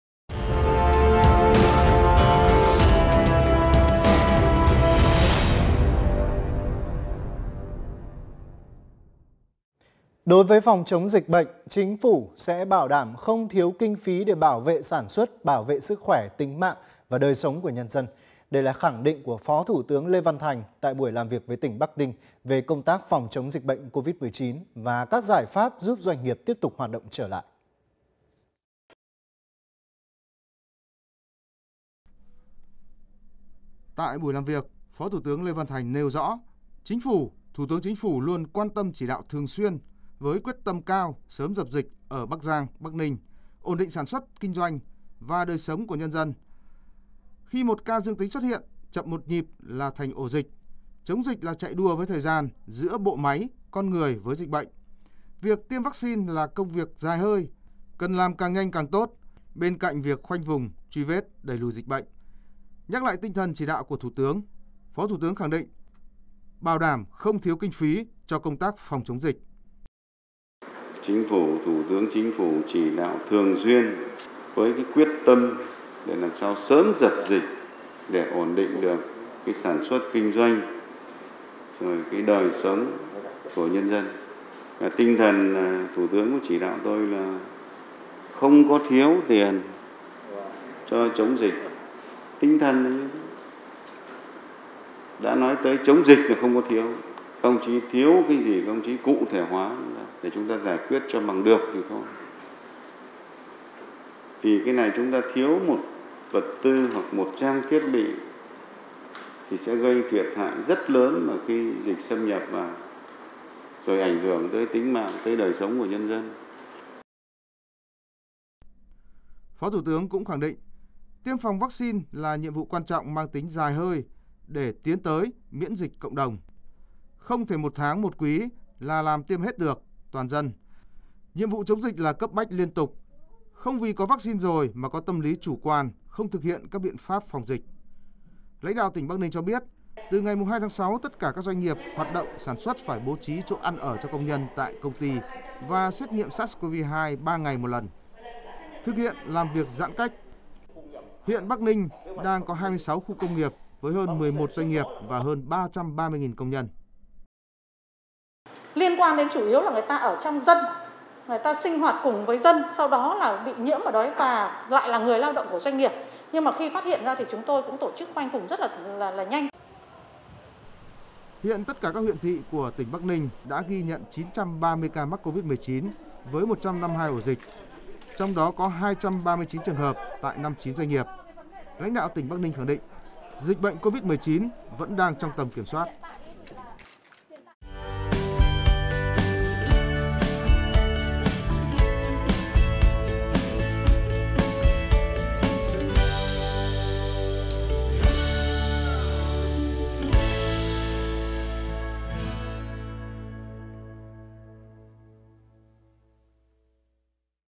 Kiểm tra phòng chống Covid-19 - Đảm bảo đủ kinh phí để chống dịch | VTV4
Đối với phòng chống dịch bệnh, Chính phủ sẽ bảo đảm không thiếu KINH PHÍ để bảo vệ sản xuất, bảo vệ sức khoẻ, tính mạng và đời sống của nhân dân. Đây là khẳng định của Phó thủ tướng Lê Văn Thành, tại buổi làm việc với tỉnh Bắc Ninh về công tác phòng chống, dịch bệnh Covid-19 và các giải pháp giúp doanh nghiệp tiếp tục hoạt động trở lại.